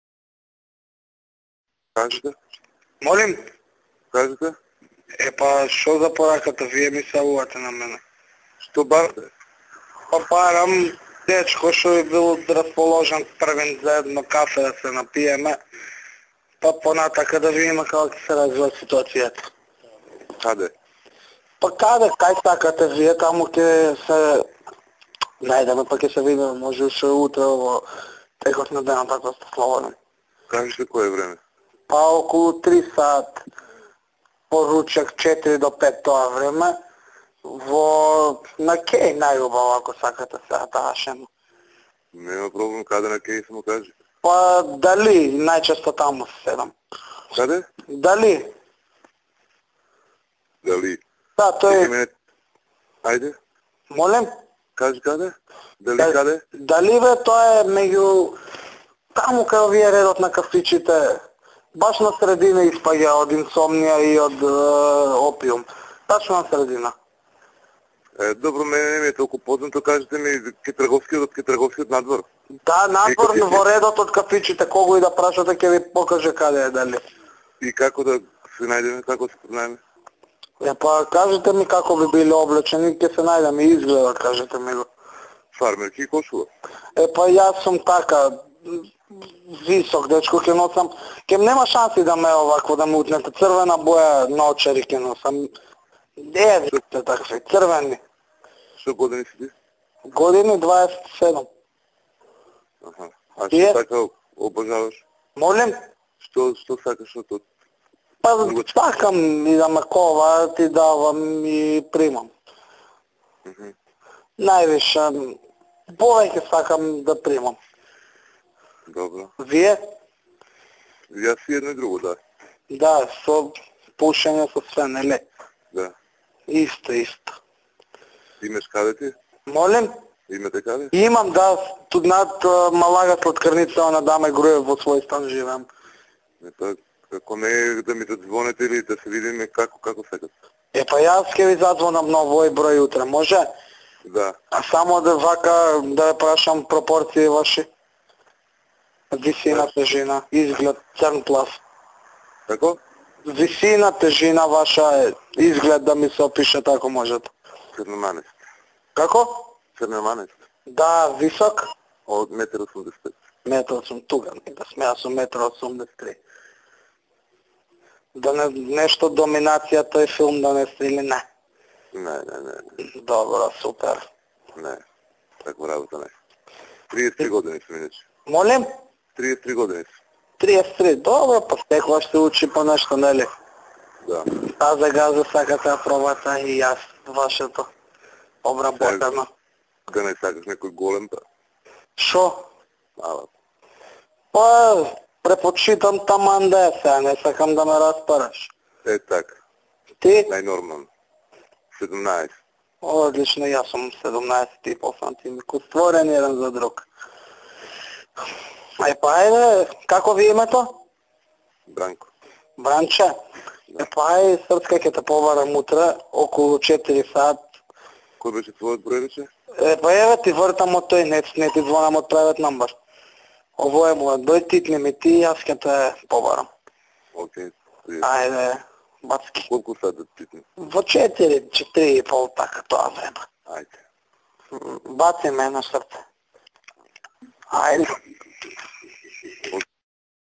Brance Taze Gaze (gay)
Telefonski razgovor megju gay i potencijalen gay:)))